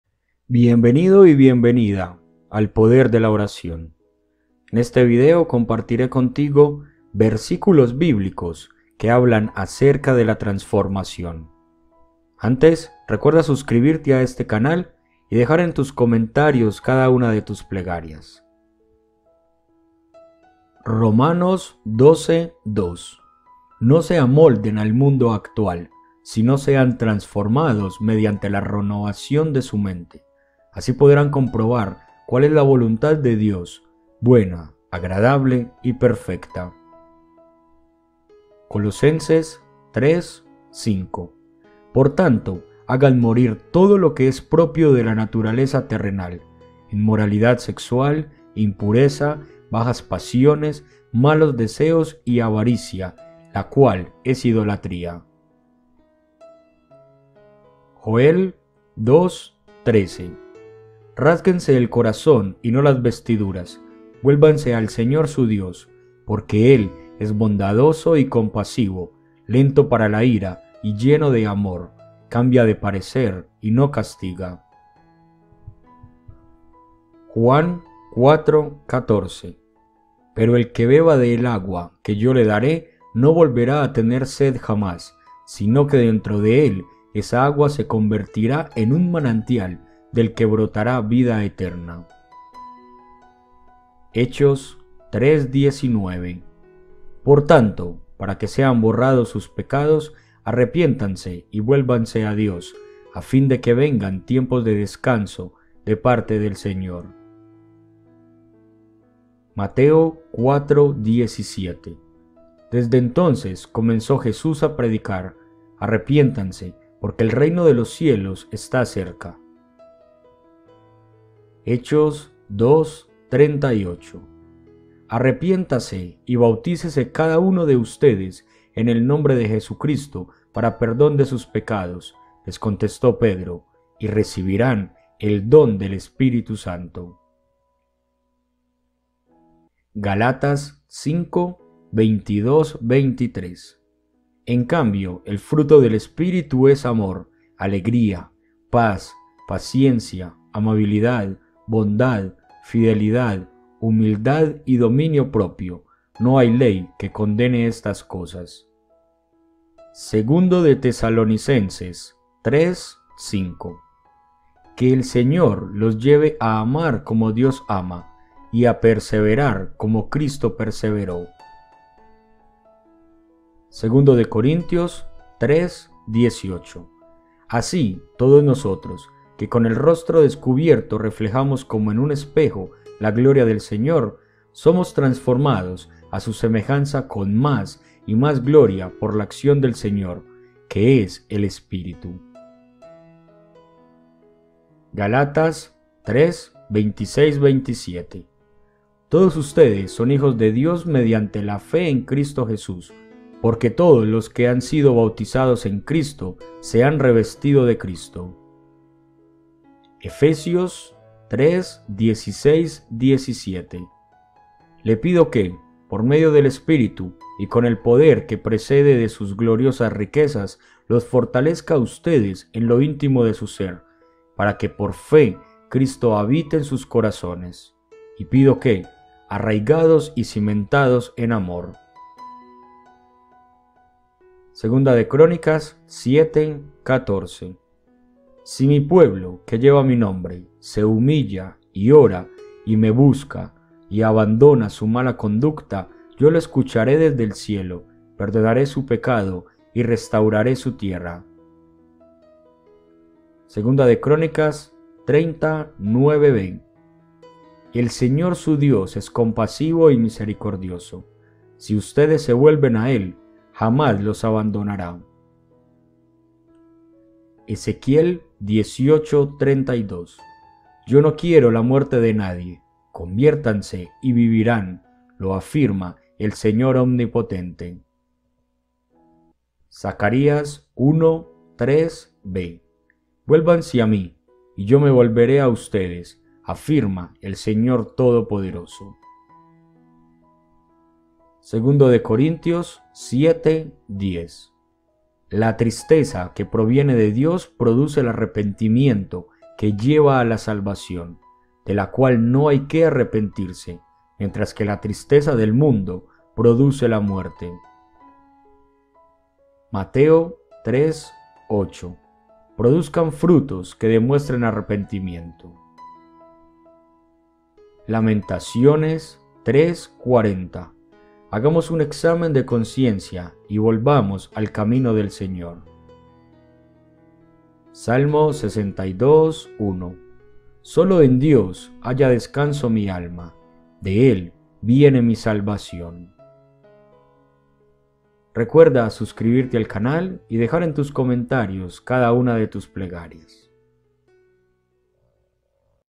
🙏🏻 Versículos de la biblia sobre la Transformación 🙏🏻 Qué dice la biblia sobre la Transformación
🙏🏻 Bienvenidos a este vídeo donde analizamos algunos de los versículos que hablan sobre la transformación.

Que dice la biblia sobre la transformación
la transformación y la biblia
mensajes de Dios sobre la transformación

Romanos 12:2
No se amolden al mundo actual, sino sean transformados mediante la renovación de su mente. Así podrán comprobar cuál es la voluntad de Dios, buena, agradable y perfecta.

Colosenses 3:5
Por tanto, hagan morir todo lo que es propio de la naturaleza terrenal: inmoralidad sexual, impureza, bajas pasiones, malos deseos y avaricia, la cual es idolatría.

Joel 2:13
Rásguense el corazón
y no las vestiduras.
Vuélvanse al Señor su Dios,
porque él es bondadoso y compasivo,
lento para la ira y lleno de amor,
cambia de parecer y no castiga.

Juan 4:14
Pero el que beba del agua que yo le daré no volverá a tener sed jamás, sino que dentro de él esa agua se convertirá en un manantial del que brotará vida eterna.

Hechos 3:19
Por tanto, para que sean borrados sus pecados, arrepiéntanse y vuélvanse a Dios, a fin de que vengan tiempos de descanso de parte del Señor.

Mateo 4:17
Desde entonces comenzó Jesús a predicar: «Arrepiéntanse, porque el reino de los cielos está cerca».

Hechos 2:38
Arrepiéntase y bautícese cada uno de ustedes en el nombre de Jesucristo para perdón de sus pecados —les contestó Pedro—, y recibirán el don del Espíritu Santo.